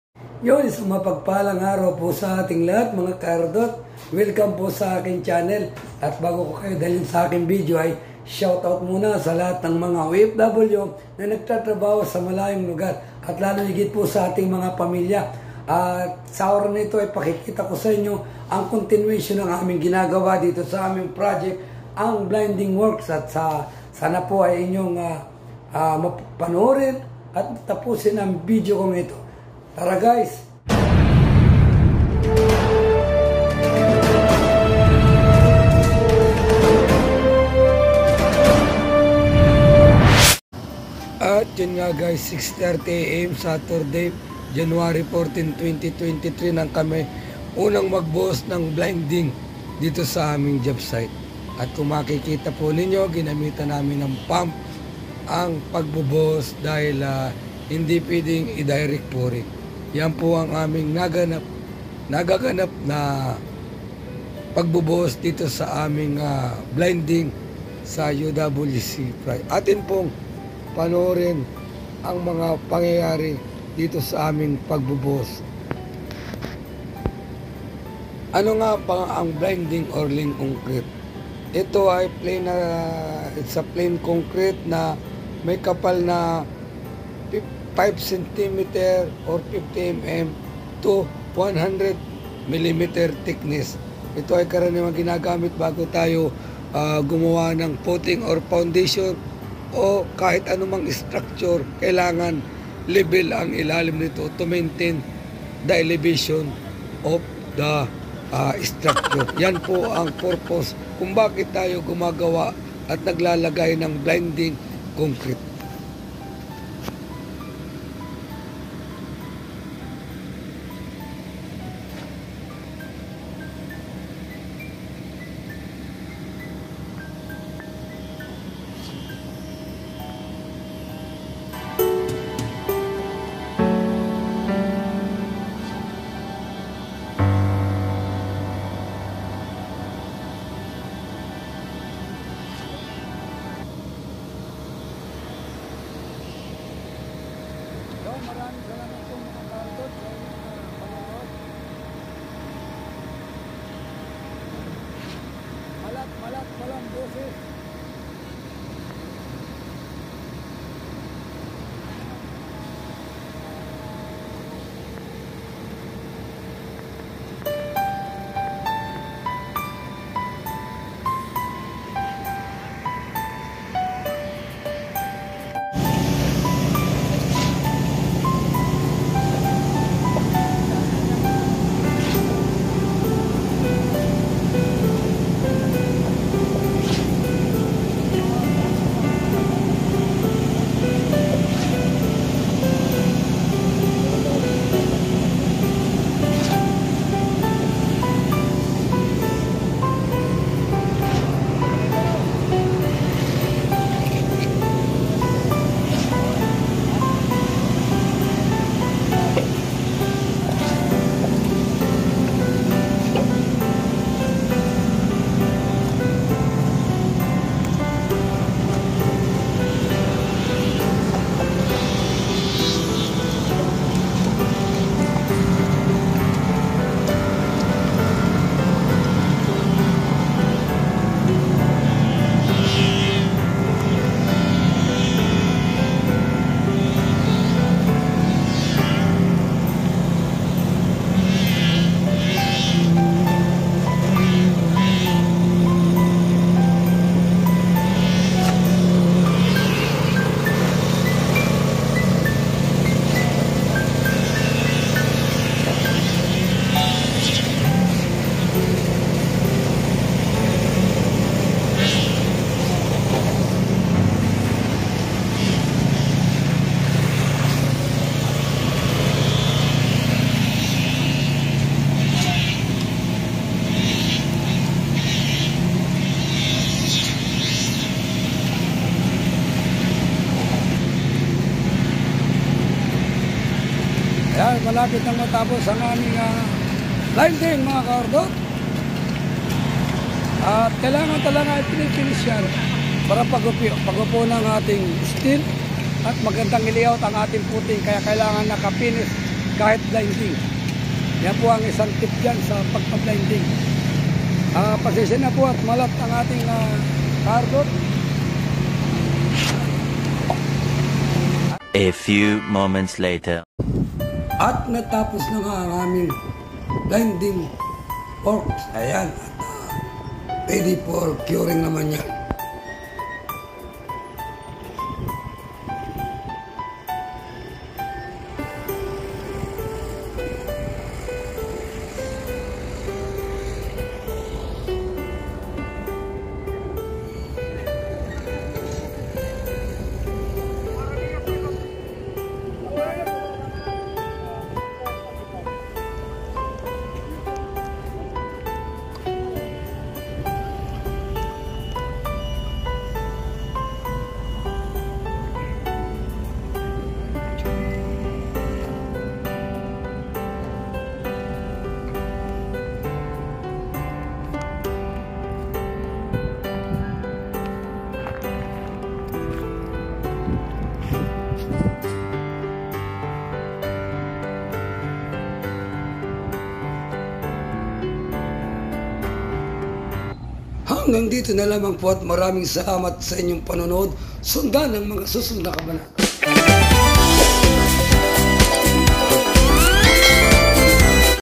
BLINDING OR LEAN CONCRETE pouring prior for foundation footing in construction
This video about the pouring of concrete fo blinding or lean concrete w/ a thickness bet 50mm to 100mm.